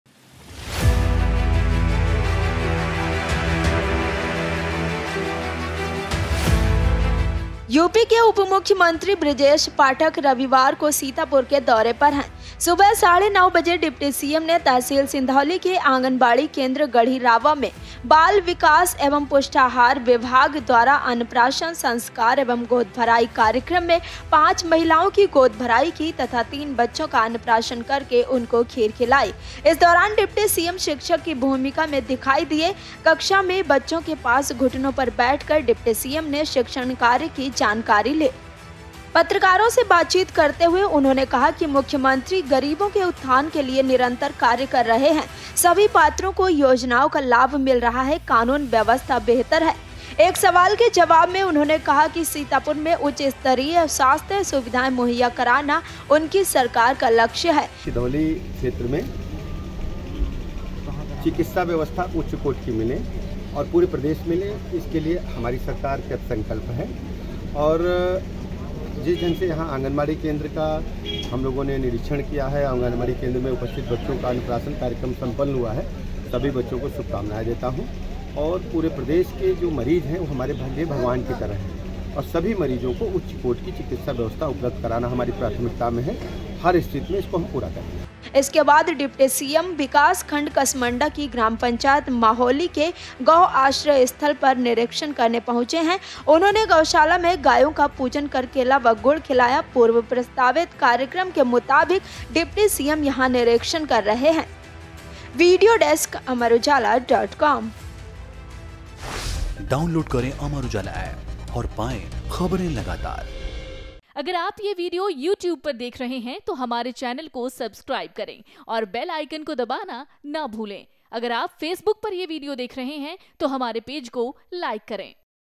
UP News: Sitapur दौरे पर Deputy CM, बच्चों को खीर खिला किया अन्नप्राशन,गौशाला में किया गायों का पूजन
Jaunpur News: यूपी के उप मुख्यमंत्री ब्रजेश पाठक रविवार को सीतापुर के दौरे पर हैं। सुबह साढ़े नौ बजे डिप्टी सीएम ने तहसील सिधौली के आगनबाड़ी केंद्र गढ़ी रावा में बाल विकास सेवा एवं पुष्टाहार विभाग द्वारा अन्नप्राशन संस्कार एवं गोद भराई कार्यक्रम में पांच महिलाओं की गोद भराई की तथा तीन बच्चों का अन्नप्राशन करके उनको खीर खिलाई...

UP Deputy Chief Minister Brajesh Pathak is on a visit to Sitapur on Sunday. At 9.30 in the morning, the Deputy CM performed the baby shower of five women in the Annaprashan Sanskar and Baby Shower program by the Child Development Services and Nutrition Department at the Anganwadi Center Garhi Rawa of Tehsil Sidhauli and fed them kheer after feeding three children.